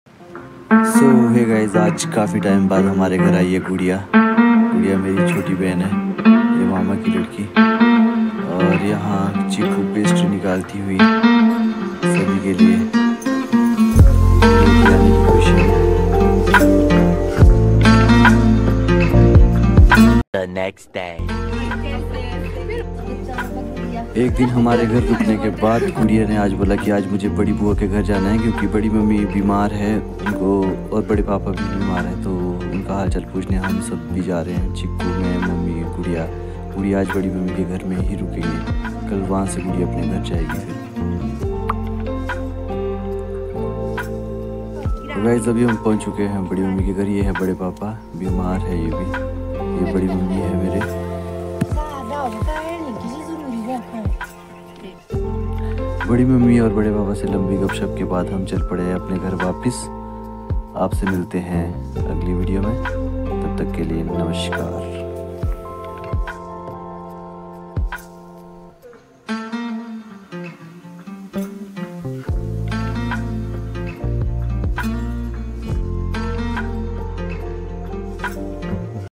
aj hmare ghr kon aya? | हम मिलने गए बड़ी मम्मी से
Aj apko milvata hu apni behn gudiya se or bde mummy bde papa se